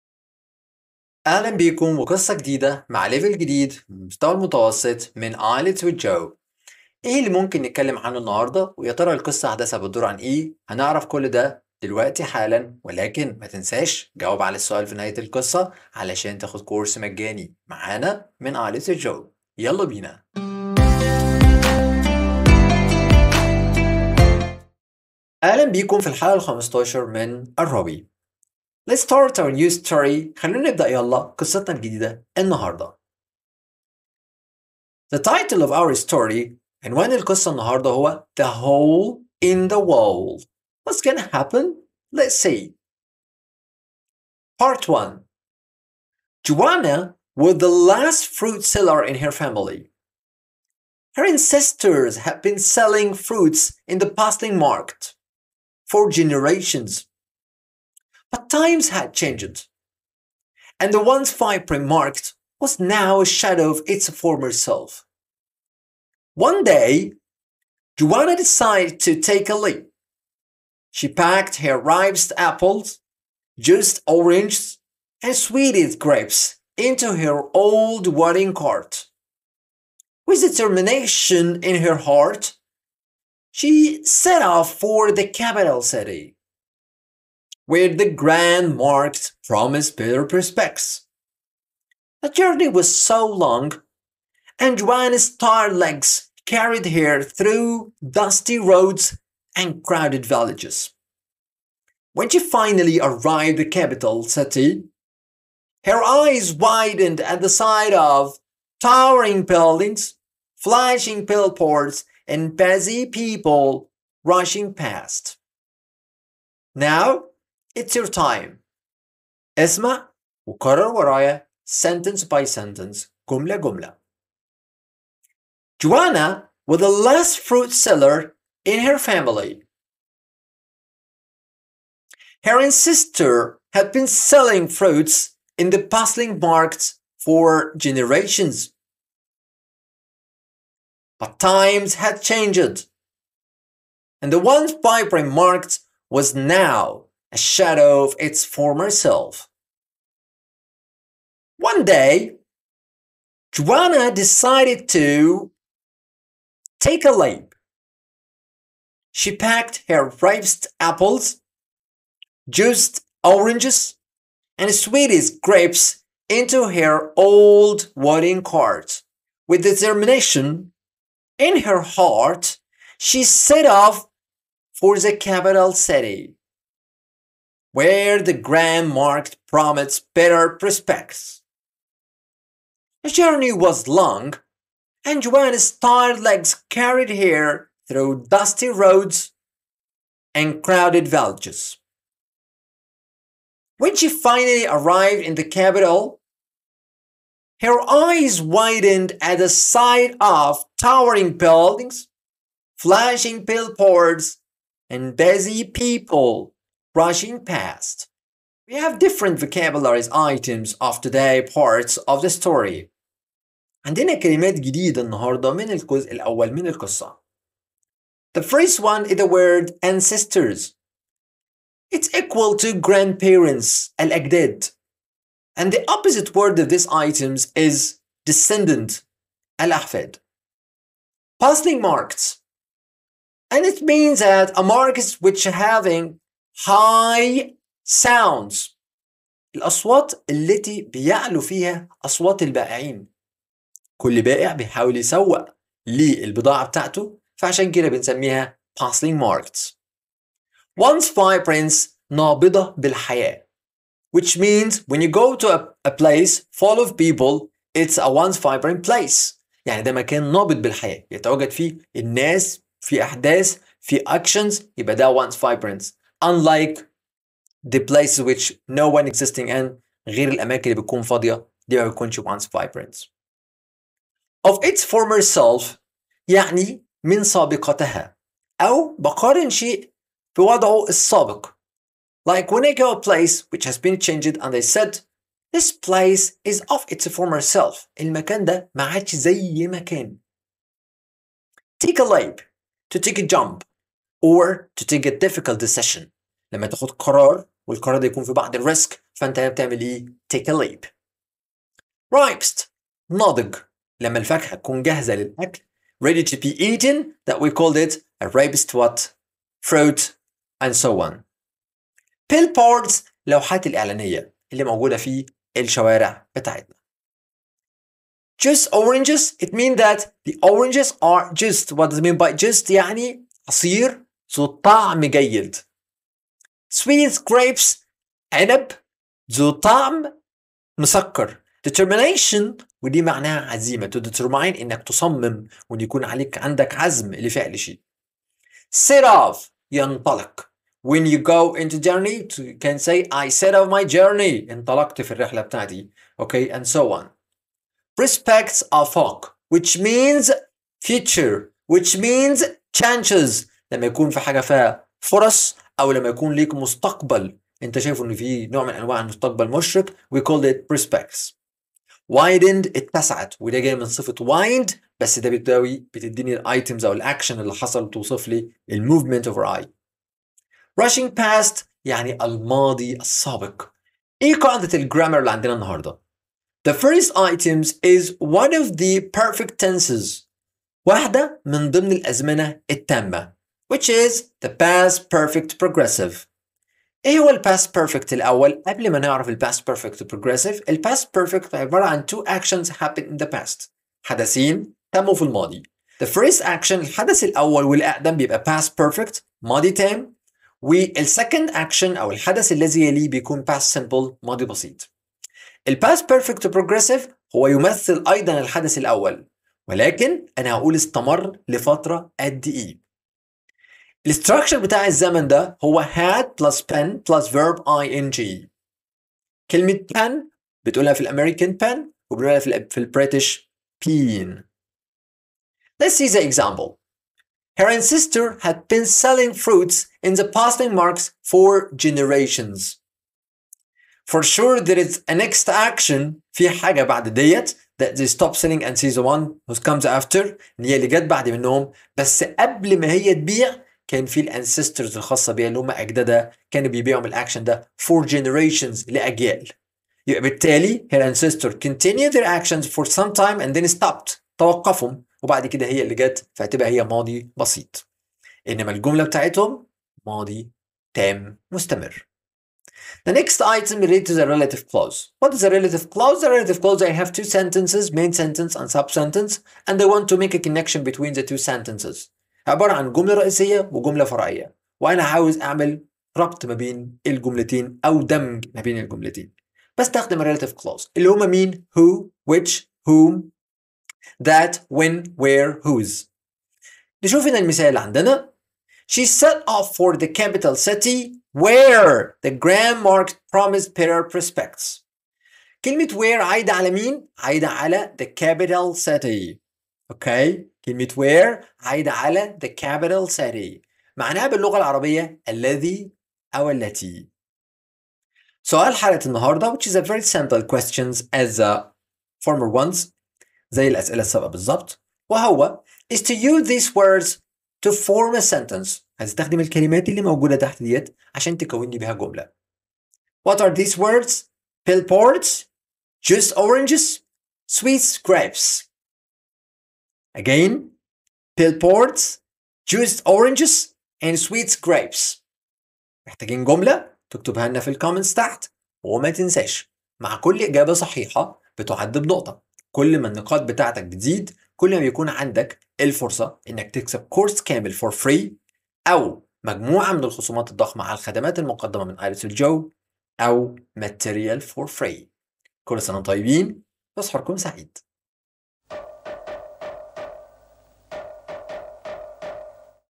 الراوي الحلقه الخامسه عشر ( المستوي المتوسط) الرزق كدعاء المطر يأتي عند الدعاء فتلقي الاستجابه.
في الفديو داه هنتكلم عن قصه جديده من قصص الراوي. الفديو يتحدث عن قصص باللغه الانجليزيه بالاضافه  الي شرح طيف يمكن ان يكون الرزق كالمطر عندما تدعو الله ان يرزقك في الاوقات الصبعه مثل دعاء المطر. بطله القصه كافحت كثيرا كل تصل الي هدفها.
الجزء الاول من القصه:
The Hole in the Wall”
Joanna was the last fruit seller in her family. Her ancestors had been selling fruits in the bustling market for generations. But times had changed, and the once-vibrant market was now a shadow of its former self.
One day, Joanna decided to take a leap. She packed her ripest apples, juiciest oranges, and sweetest grapes into her old wooden cart. With determination in her heart, she set off for the capital city, where the grand market promised better prospects.
The journey was long, and Joanna’s tired legs carried her through dusty roads and crowded villages. When she finally arrived in the capital, her eyes widened at the sight of towering buildings, flashing billboards, and busy people rushing past.